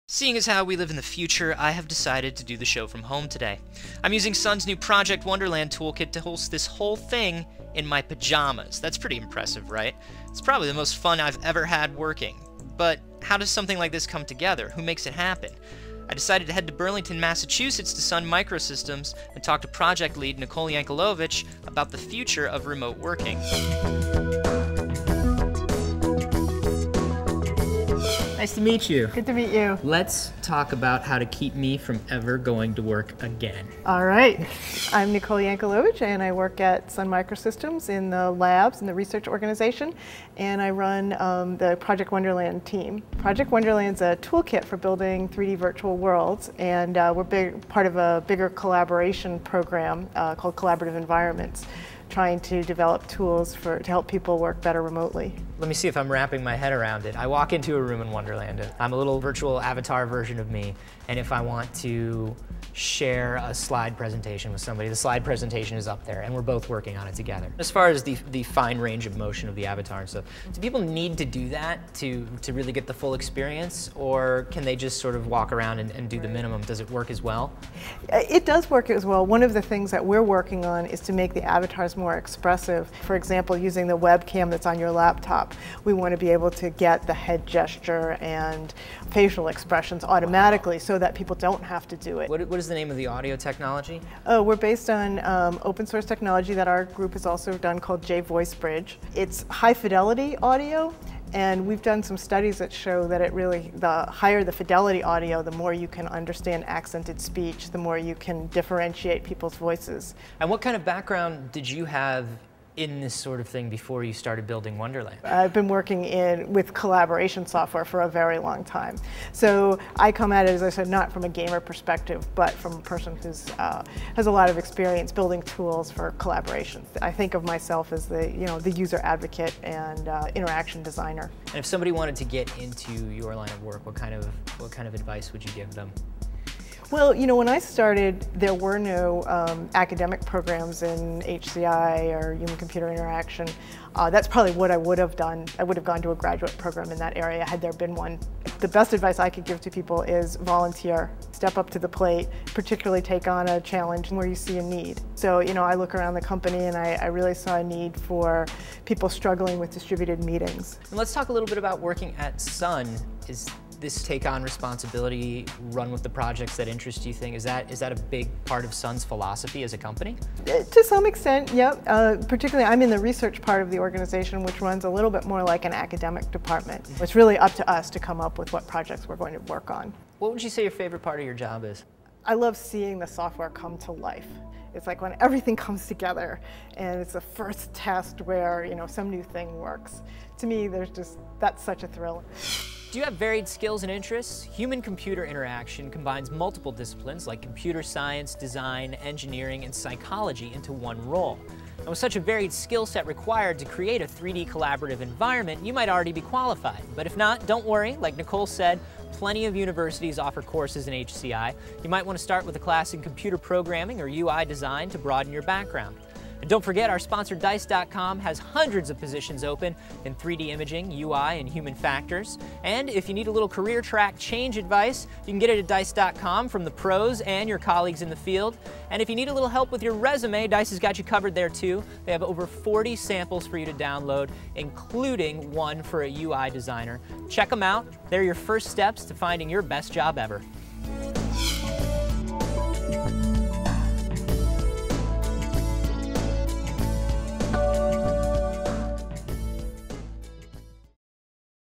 Meet Nicole Yankelovich, Project Lead at Sun Microsystems
If youre in tech, then you know that working remotely is a new phenomenon sweeping the workforce.  Thats where Nicole comes in. She leads Project Wonderland, a Sun Microsystems toolkit for building 3D virtual worlds that help people work remotely.  Someday we may all gather in an all-staff meetings in a world created by Nicole.